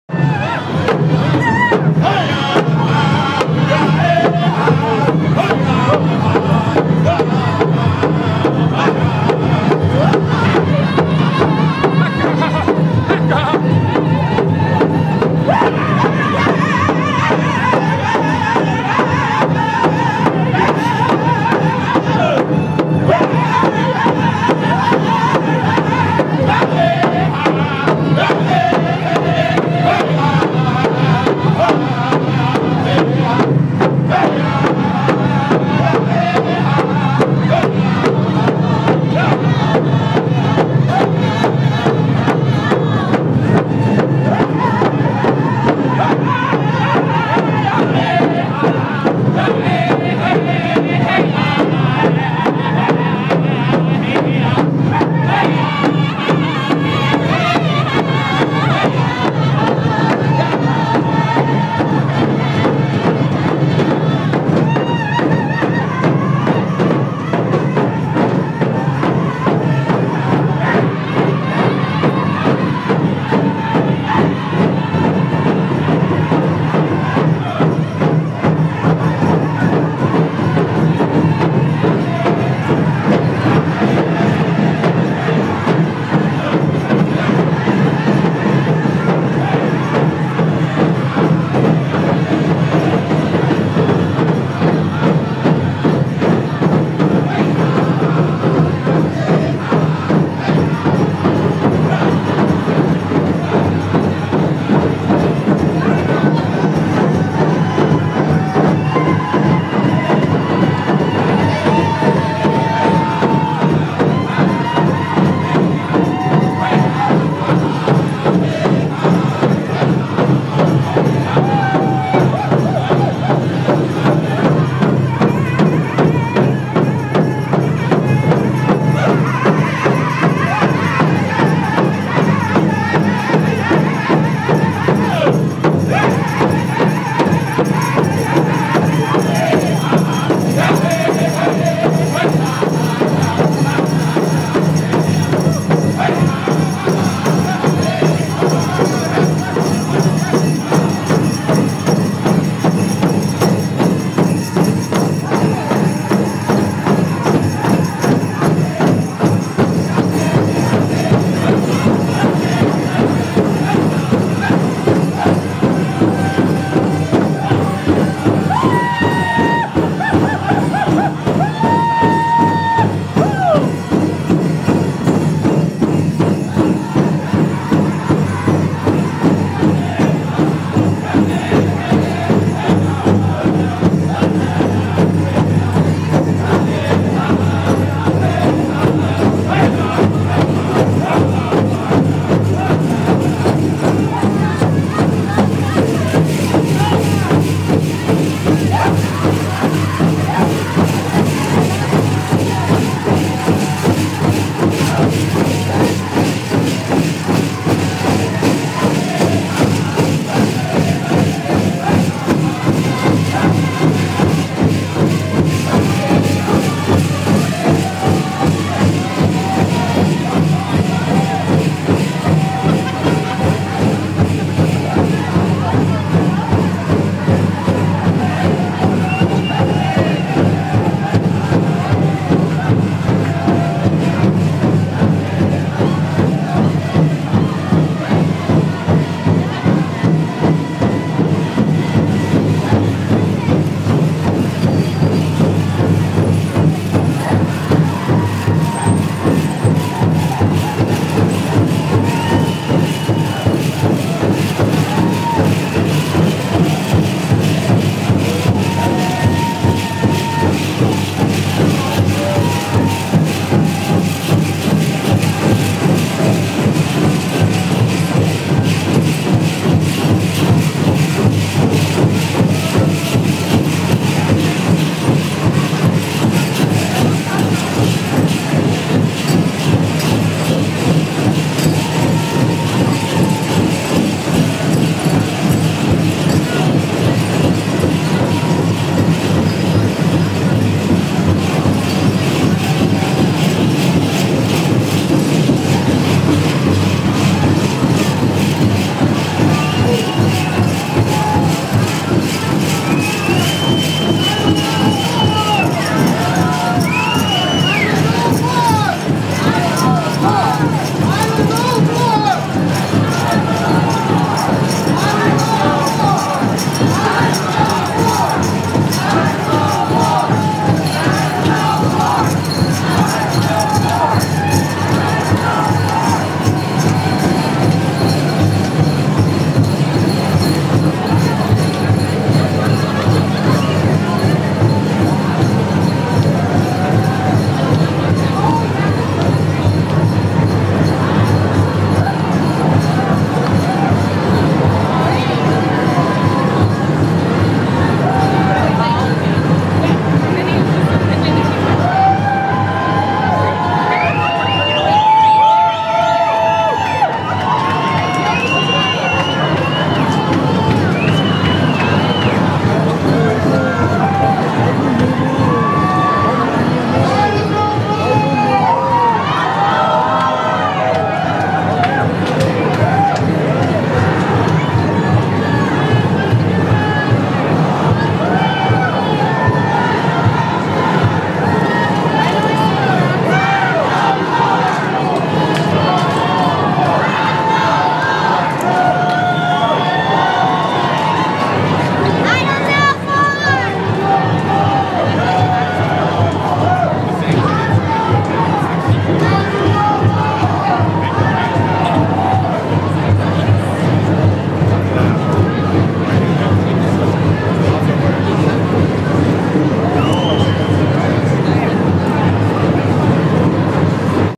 Idle No More WEM Grand Entry - Part Three
Dancers and protesters make their way through West Edmonton Mall at the Idle No More Grand Entry at West Edmonton Mall on January 13, 2013 - this one was shot on the main level.